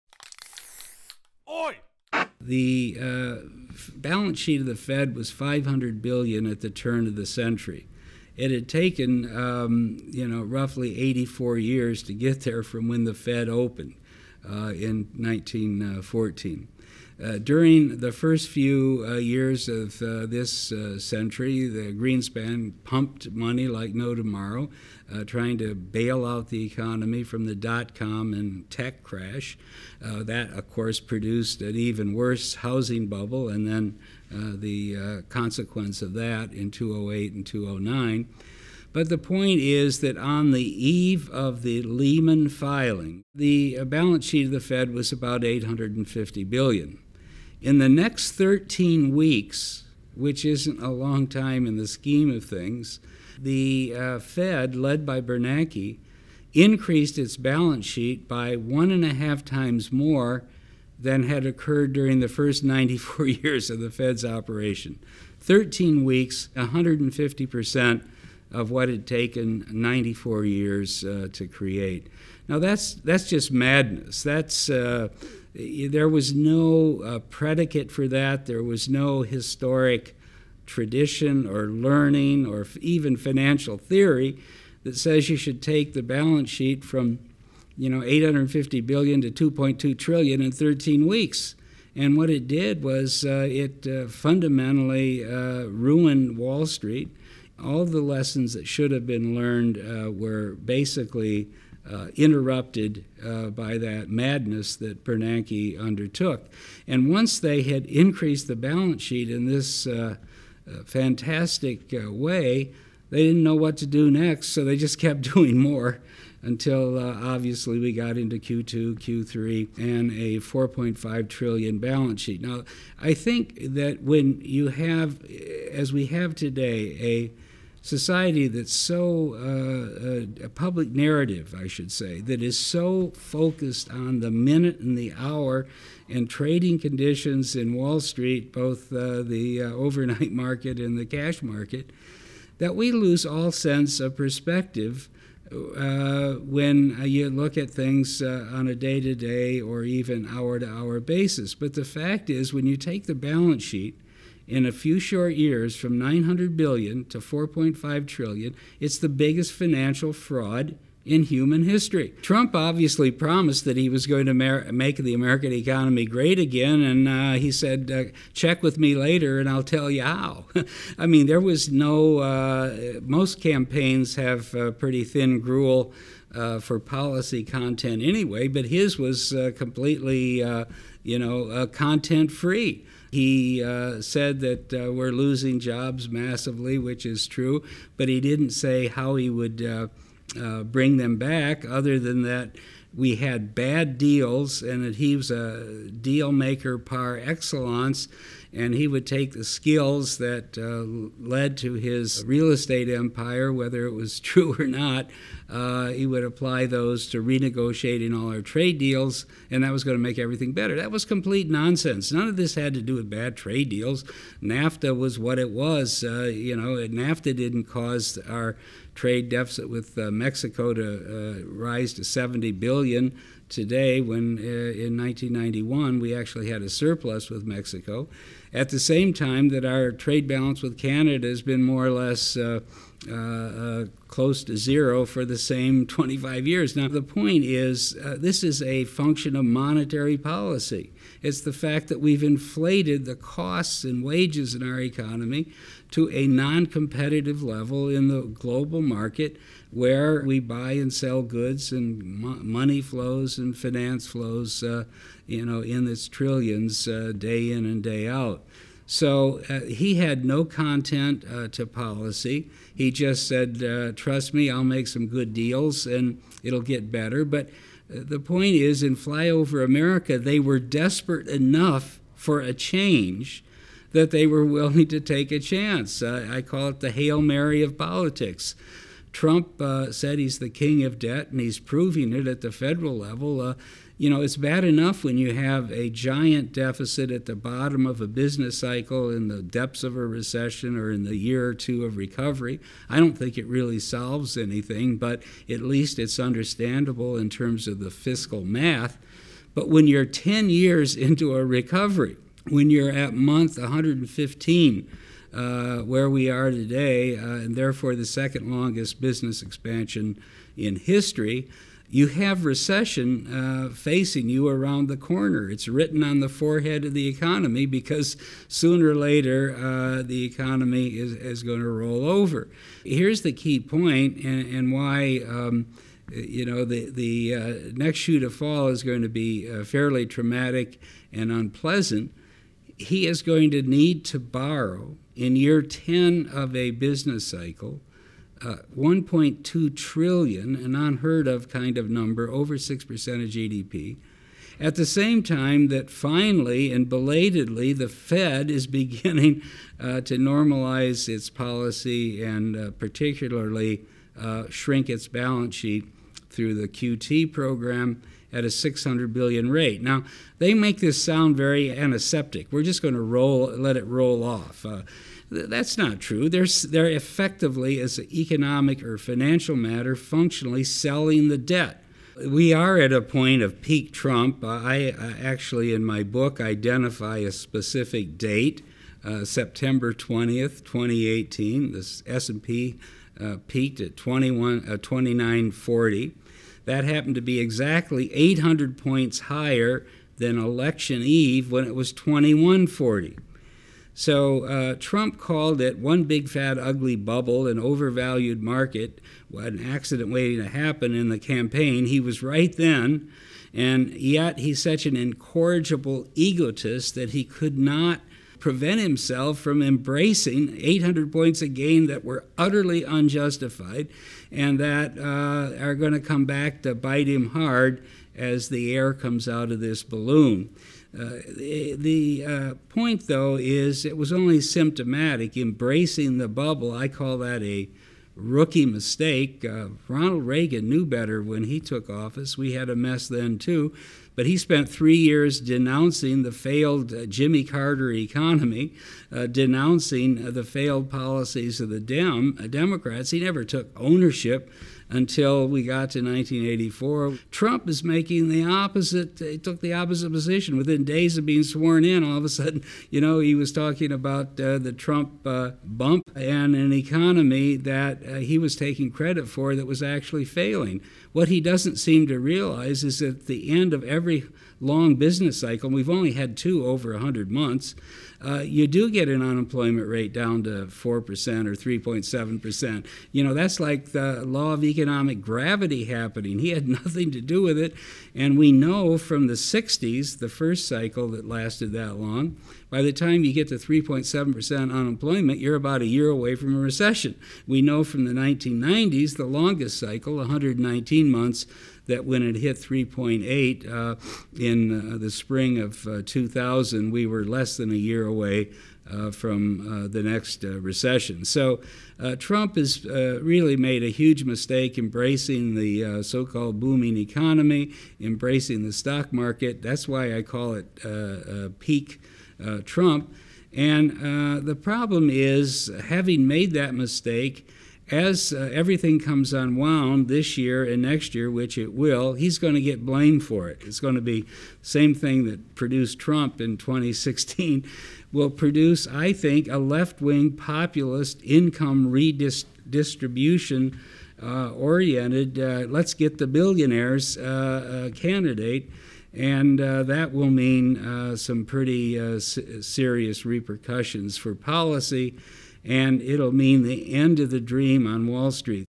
🔴 How the Fed Paved the Way to Trump (w/ David Stockman)
David Stockman, the former director of the Office of Management and Budget under Ronald Reagan, argues that the Fed’s decision to rapidly expand its balance sheet in response to the financial crisis set the table for a breakdown in global economics and politics. Stockman argues that the knock-on effects from the Fed’s decade-old decision could have significant consequences. This clip is excerpted from a video published on Real Vision on February 25, 2019 entitled “The Economic Danger Posed by Trump.”